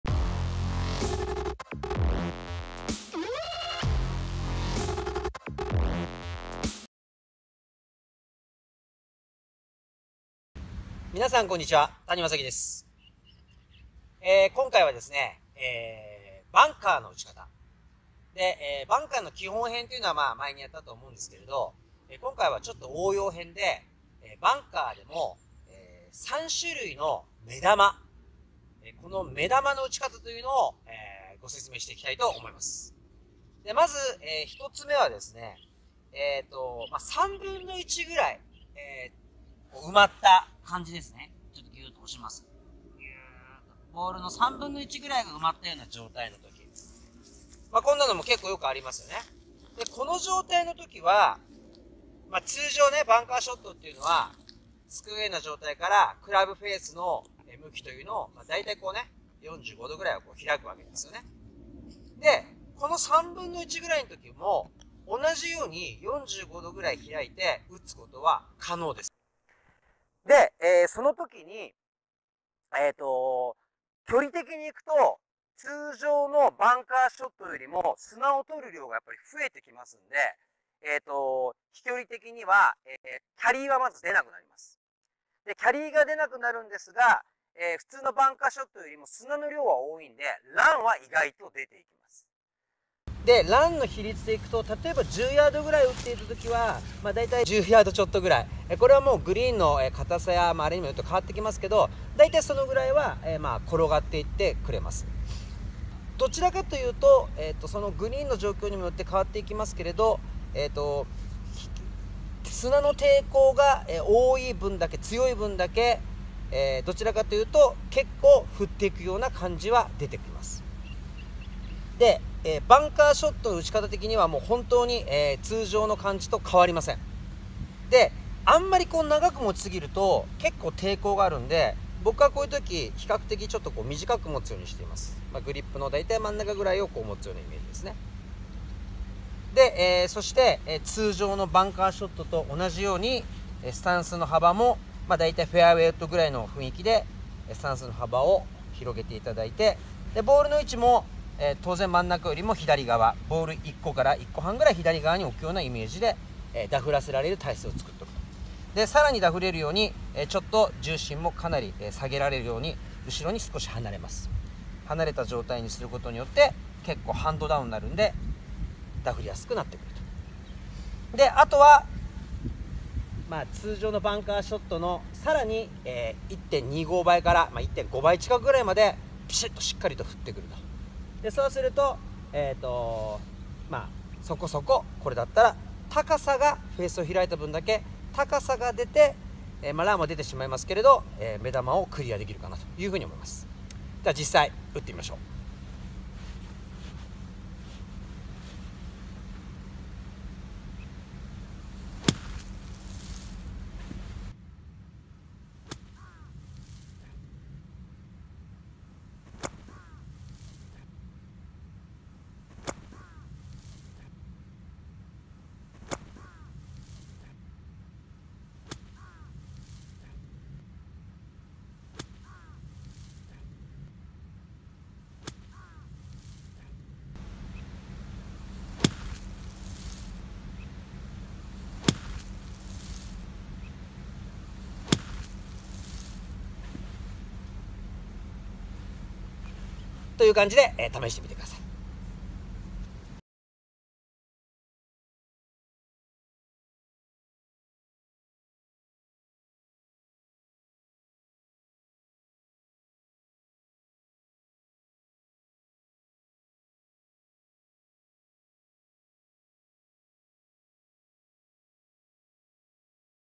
【バンカー 目玉の打ち方】ボールが砂に1/3埋まっている場合。賞金王のコーチがレッスン！
まだバンカーショットの打ち方を見ていない方はこちらをご覧ください！

谷将貴の新書が発売開始！
【ゴルフ きれいなインパクトの作り方】
購入はこちらから

【撮影場所】
京葉カントリー倶楽部